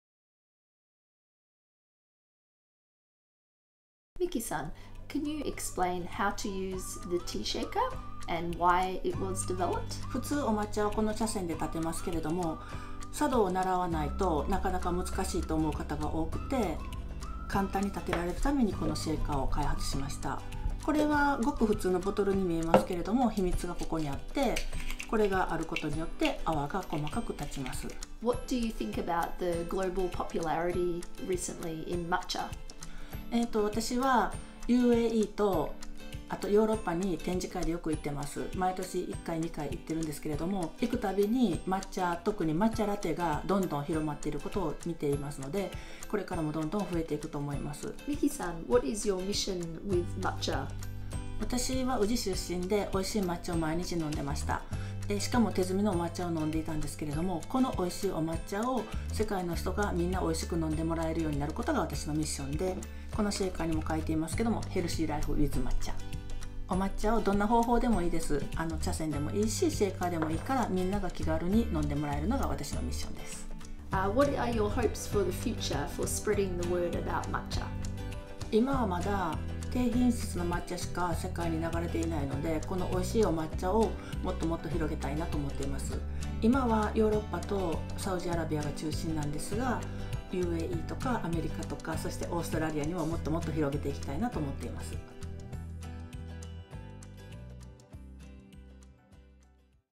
Want to know how to make an easy cup of Matcha? No bamboo whisk required.
The Tea Shaker was invented in Japan as an convenient, fuss free way to make a cup of Matcha easily.

Its unique lid allows the Matcha to aerate, leading to tasty frothy Matcha to enjoy.

Traditionally a Bamboo Whisk (Chasen) is used to make Matcha however the Tea Shaker allows people around the world to enjoy Matcha Green Tea without needing to know formal Japanese tea ceremony.